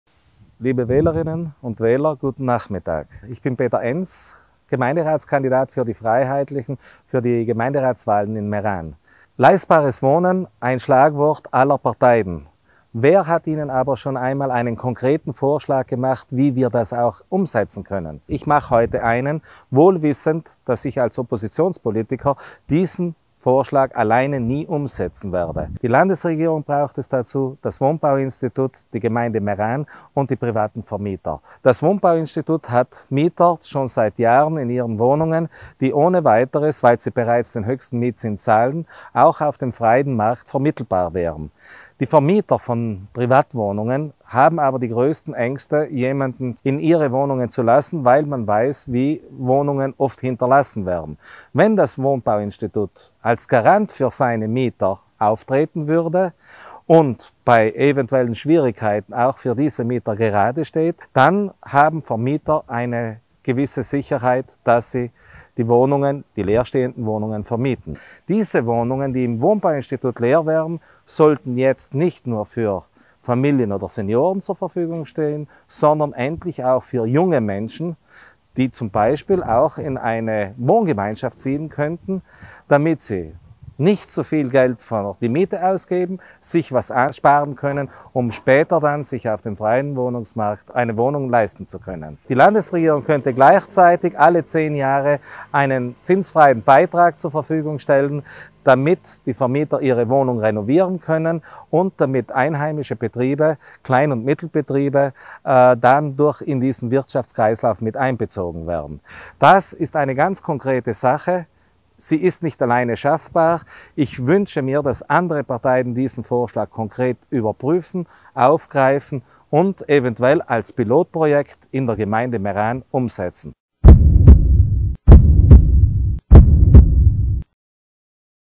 Wohnen leistbarer machen: Pilotprojekt „Sorgloses Vermieten“ für Meran
Die Preise für Wohnraum in Meran steigen seit Jahren schier unaufhörlich. Mit dem von uns Freiheitlichen geforderten Modell des „sorglosen Vermietens“ könnten neue Anreize zur Vermietung von Wohnungen geschaffen und den Wohnungseigentümern damit Schutz vor Mietausfällen und Sachschäden geboten werden.

Damit liefern wir einen konkreten Lösungsansatz für das Problem des hohen Wohnungsleerstandes (über 2.000 Wohnungen), um das Wohnen für möglichst viele Meraner und Meranerinnen wieder leistbarer zu machen.